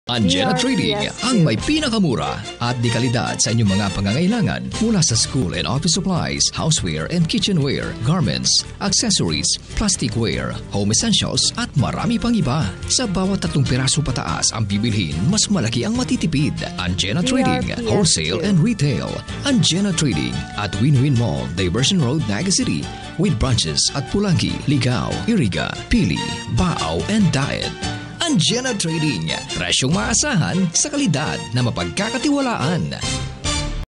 Angena Trading Radio Ad 2022 34s (Naga City)
This is the 2022 Provincial Radio Ad for Angena Trading in Naga City.

DRPS Two 2306: EO Executive Optical Radio Ad 2H 2022 15s